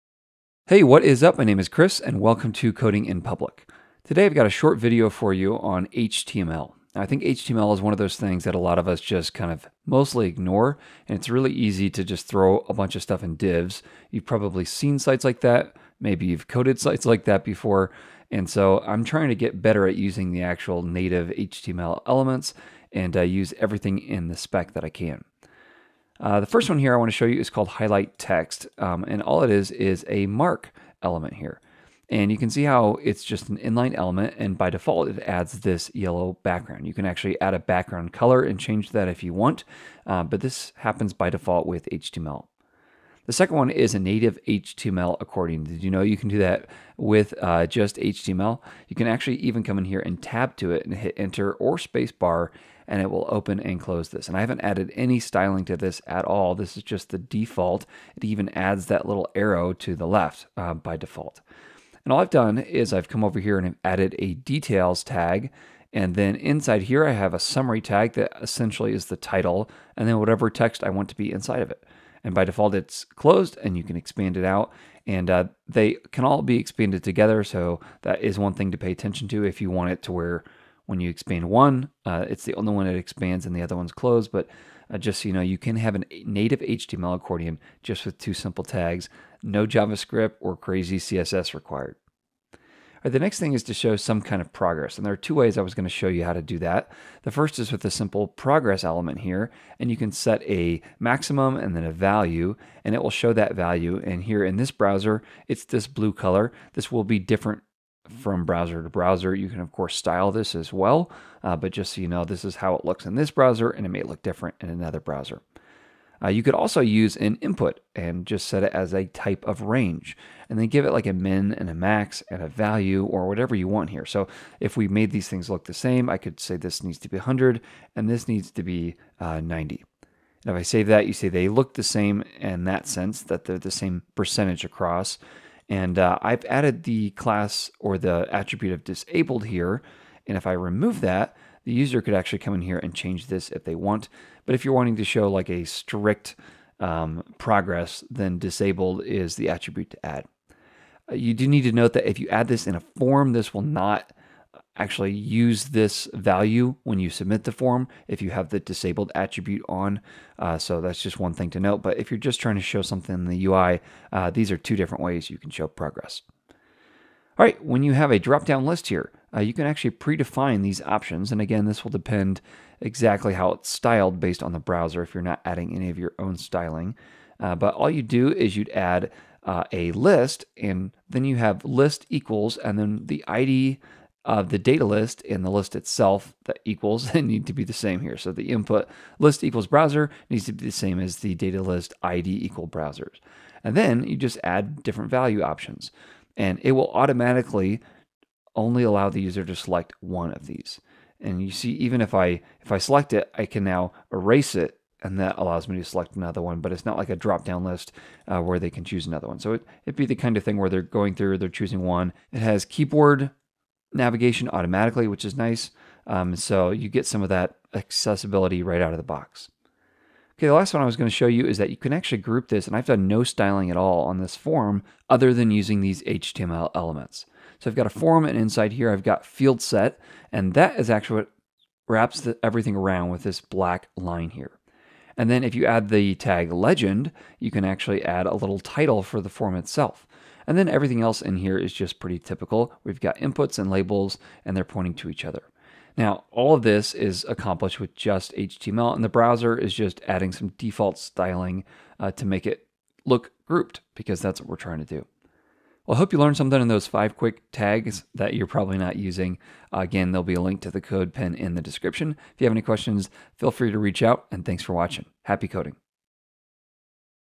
5 HTML Tags You’re Probably Not Using
Native HTML includes so many unused elements which have default accessibility, styling, and more. In this short video, I'll show you 5 HTML tags you’re probably trying to duplicate with CSS and/or Javascript but that are part of the HTML spec.